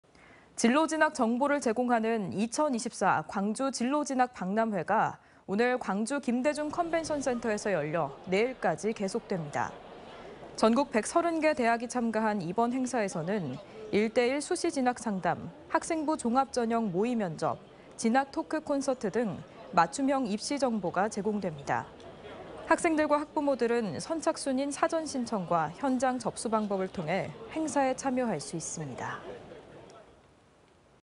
2024 광주진로진학박람회 오늘부터 열려
진로진학 정보를 제공하는
'2024 광주진로진학박람회'가
오늘(19) 광주 김대중컨벤션센터에서 열려
내일까지 계속됩니다.

전국 130개 대학이 참가한 이번 행사에서는
1대 1 수시 진학 상담,
학생부종합전형 모의면접,
진학 토크 콘서트 등
맞춤형 입시 정보가 제공됩니다.

학생들과 학부모들은
선착순인 사전 신청과
현장 접수 방법을 통해
행사에 참여할 수 있습니다.